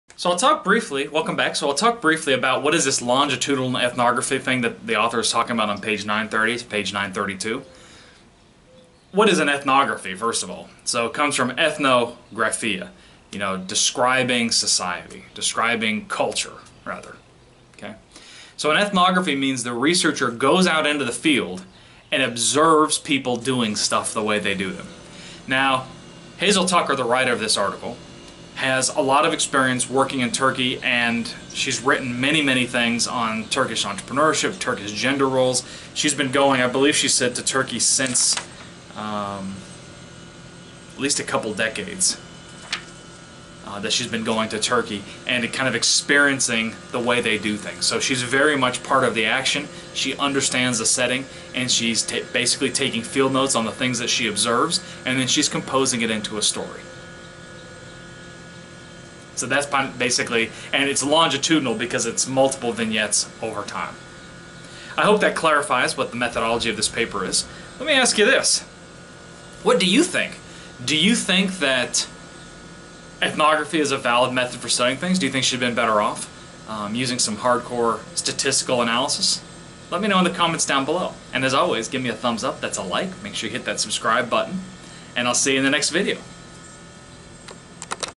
What is a Longitudinal Ethnography?
In this video I discuss the methodology used to explore peasant entrepreneurship - the longitudinal ethnography.